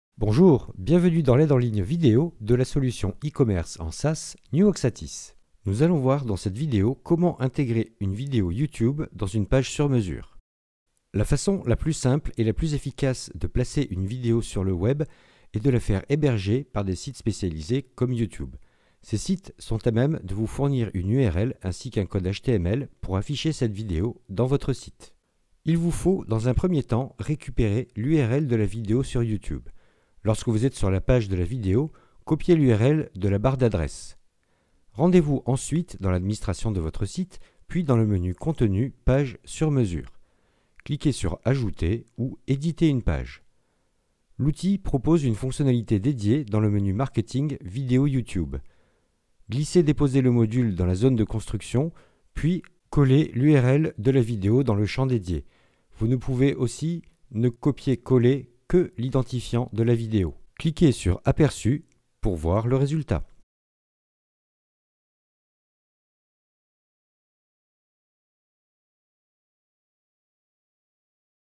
Aide en ligne en vidéo NewOxatis - Intégrer des vidéos YouTube dans une page sur mesure
Aide en ligne en vidéo de la solution e-Commerce en SaaS NewOxatis - Intégrer des vidéos YouTube dans une page sur mesure